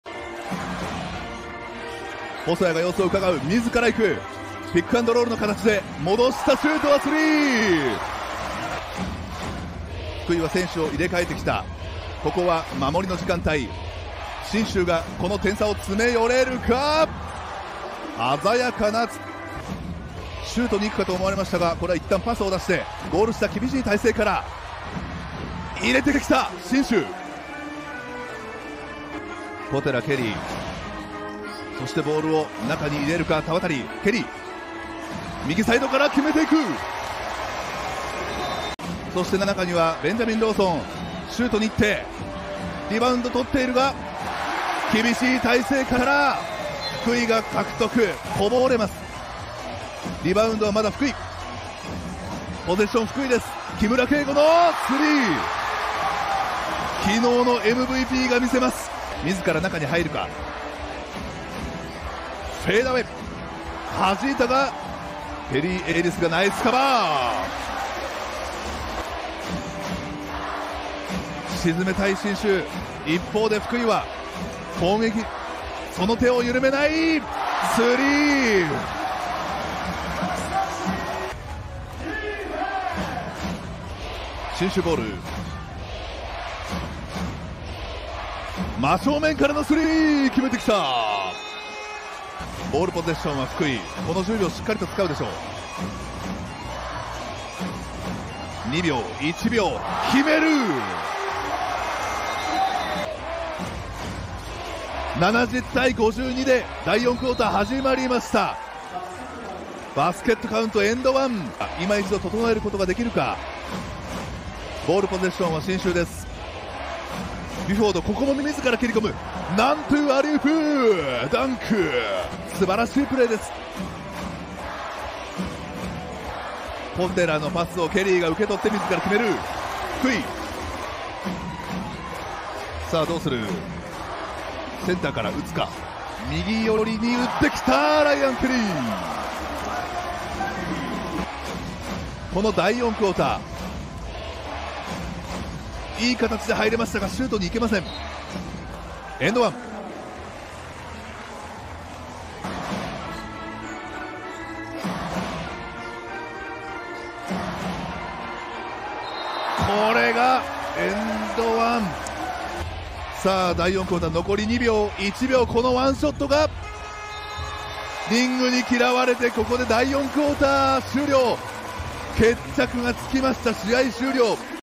【ハイライト】福井ブローウィンズvs信州ブレイブウォリアーズ｜B2第1節GAME2｜10.06.2024 プロバスケ (Bリーグ)
Watch the Game Highlights from Fukui Blowinds vs. Shinshu Brave Warriors, 10/06/2024